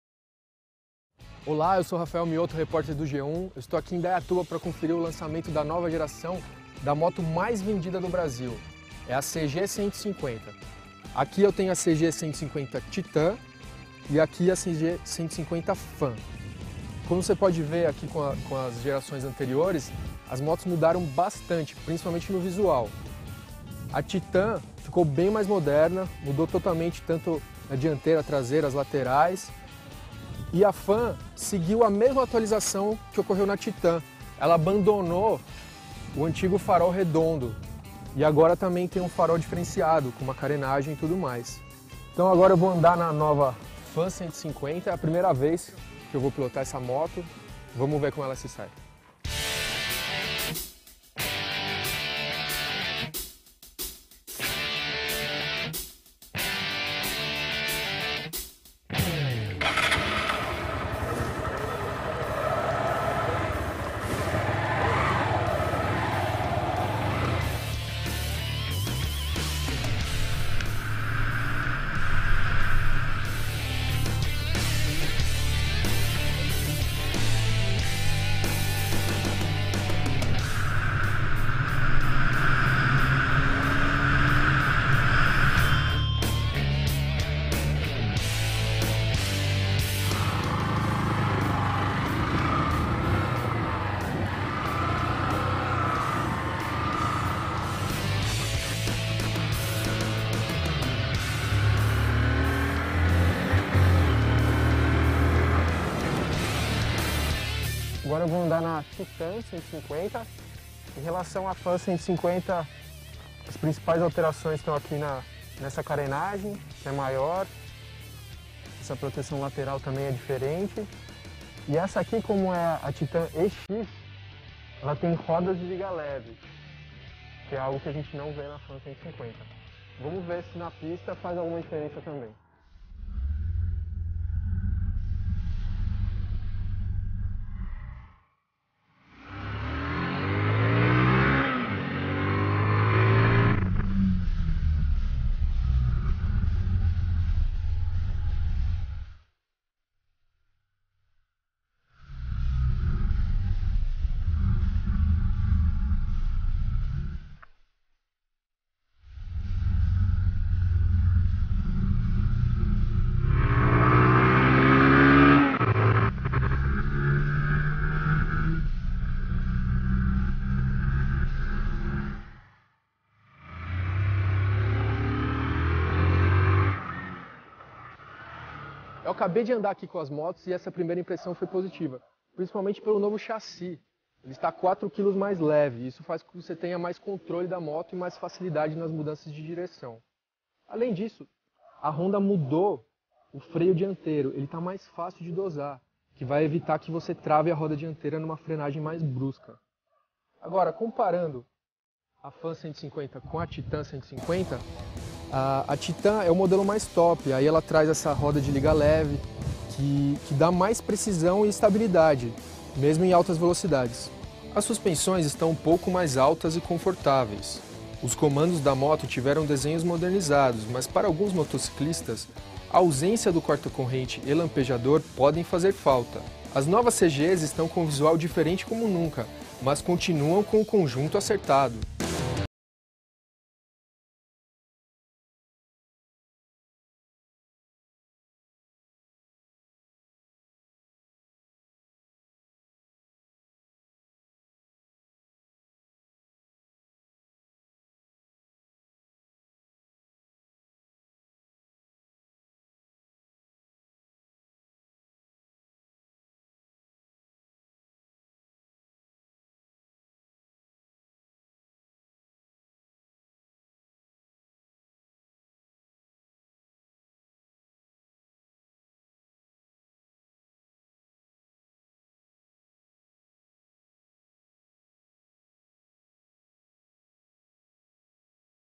Titan 150 - Confira a avaliação das novas CG 150 Fan e Titan
Titan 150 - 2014 - G1 fez avaliação avaliação das novas CG 150 Fan e Titan .